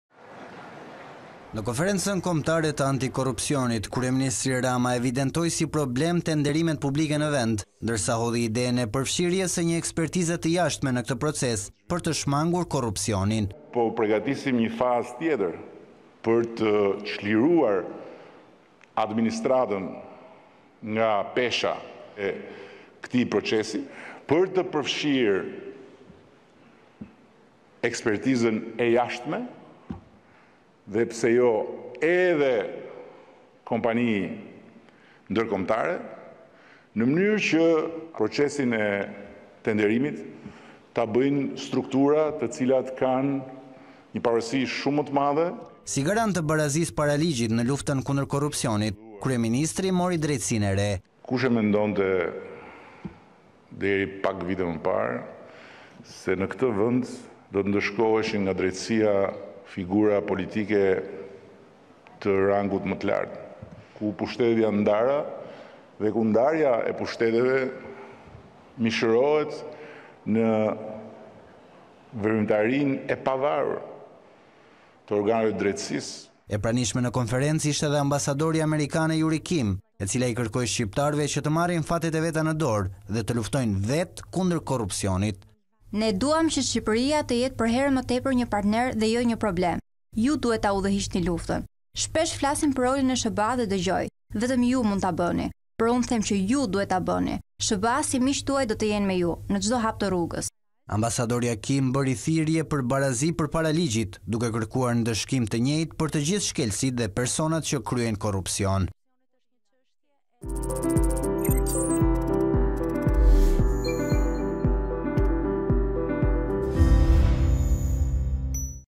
Rama: Ekspertizë e jashtme për tenderat, të jetë strukturë e pavarur
Në Konferencën Kombëtare të Antikorrupsionit, kryeministri Edi Rama evidentoi si problem tenderimet publike në vend, ndërsa hodhi idenë e përfshirjes së një ekspertize të jashtme në këtë proces për të shmangur korrupsionin.